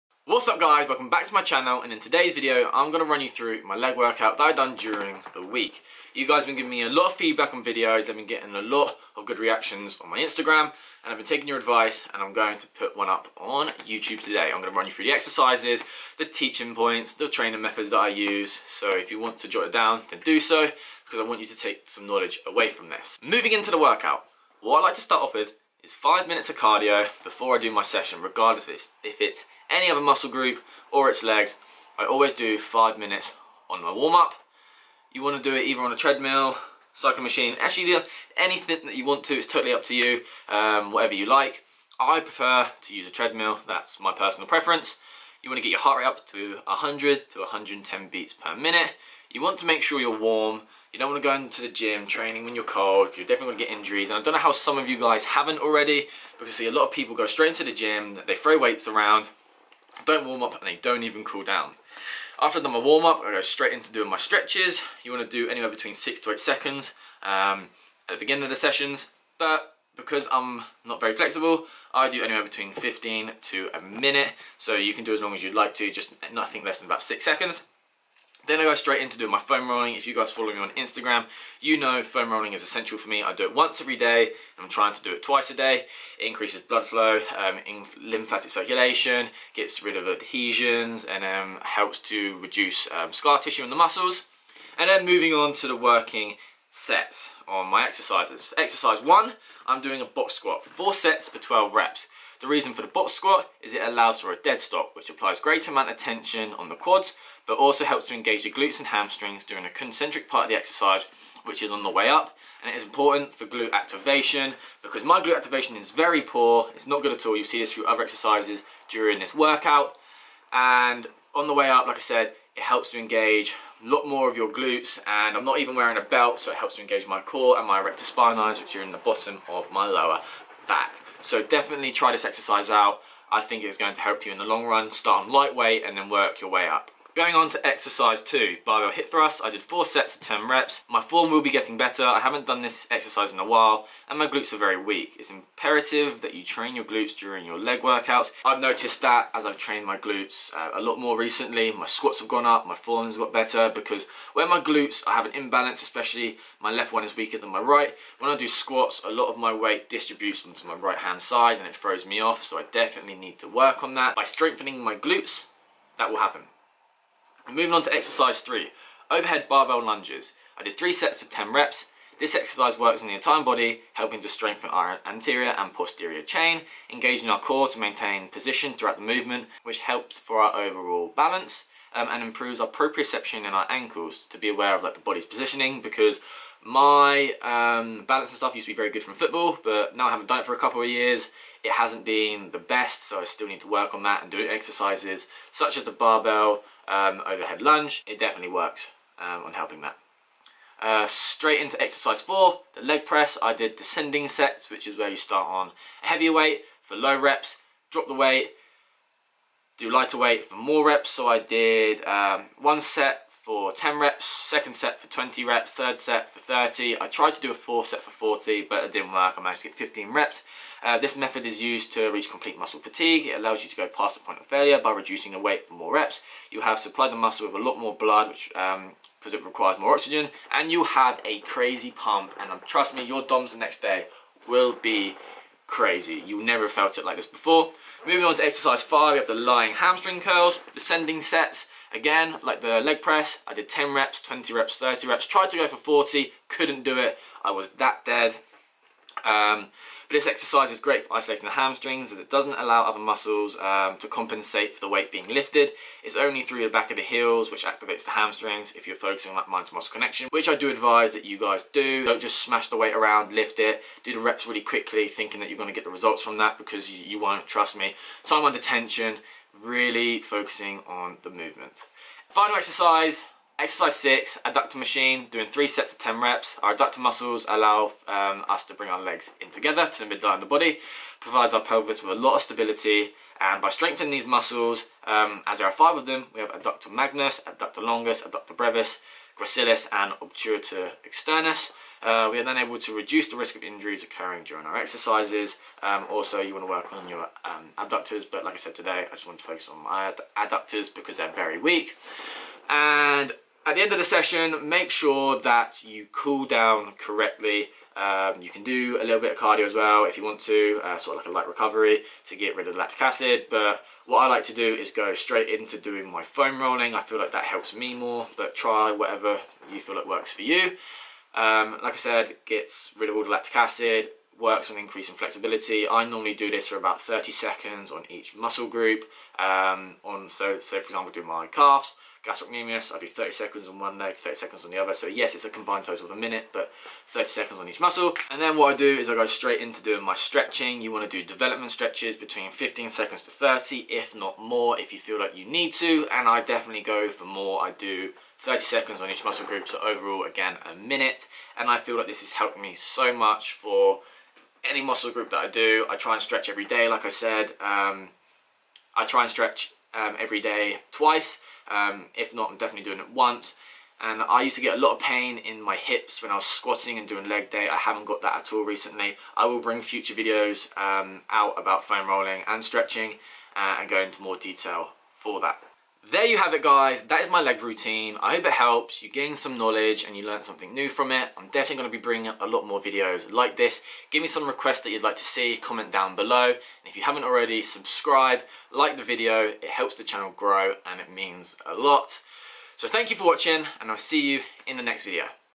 Taking You Through My Leg Workout
Thank you guys for watching I hoped this helped you out and you gained something new and use these exercises in your next workout.